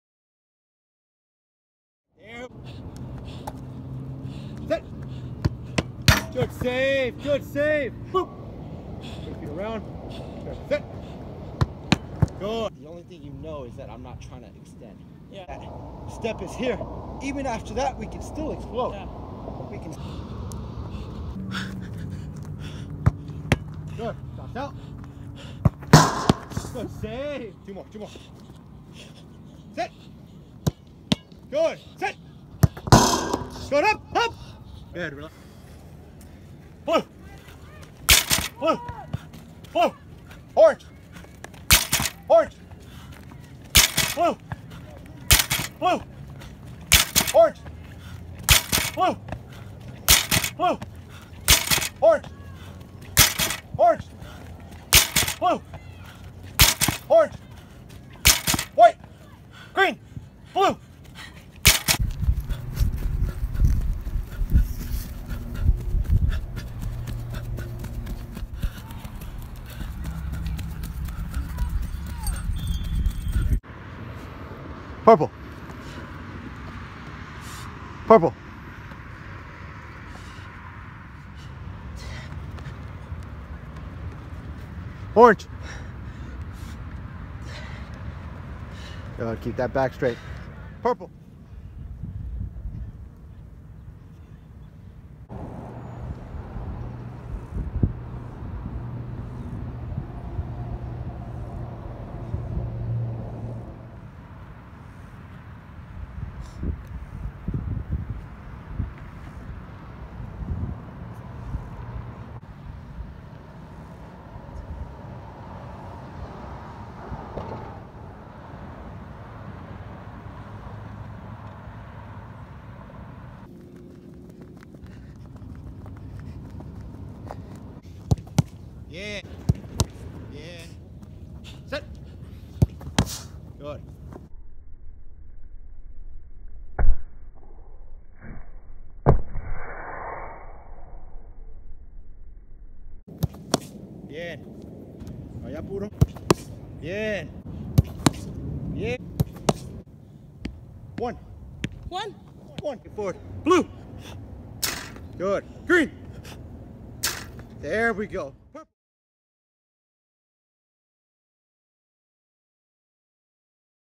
MSF KEEPERS Goalkeeper Training with UCLA goalkeeper, Idalia Serrano
“If it doesn’t challenge you, it won’t change you.” MSF Keepers is fortunate to have UCLA goalkeeper, Idalia Serrano train during the offseason here. Idalia has tremendous footwork and awareness in goal, as well as extraordinary strength and reaction time! She is an amazing young goalkeeper with great potential.